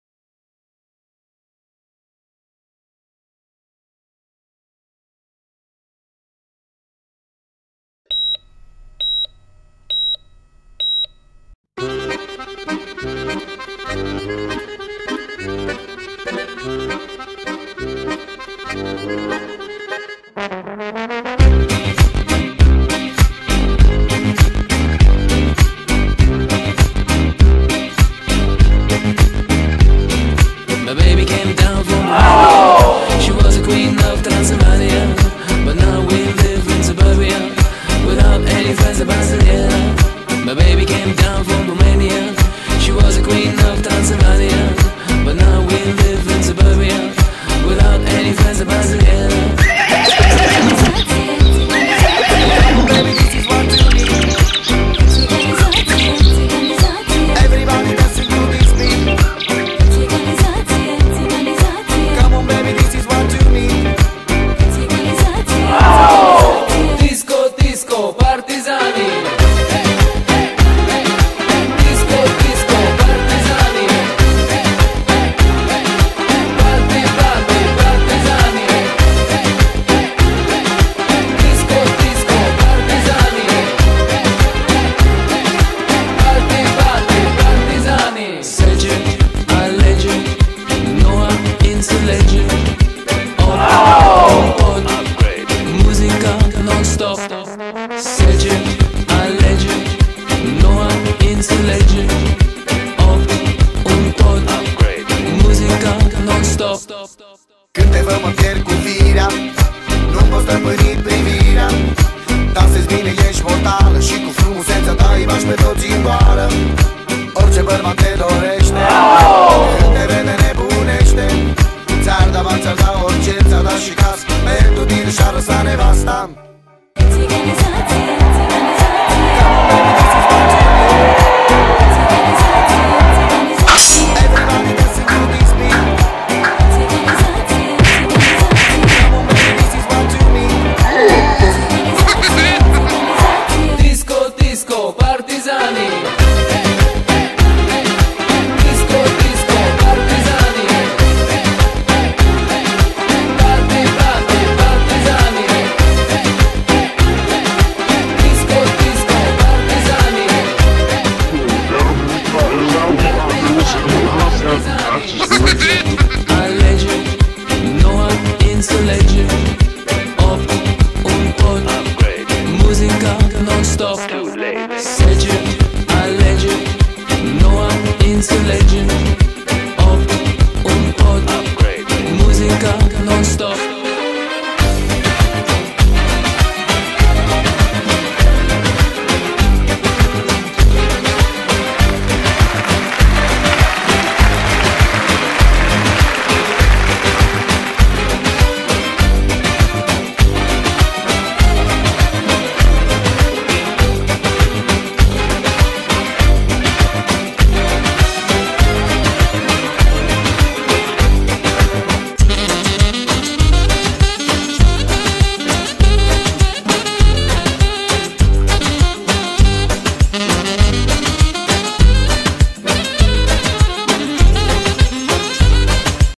Ковер-самолет
Я с мужем летим над Отелем Амара дольче Вита в Текирова/Кемер.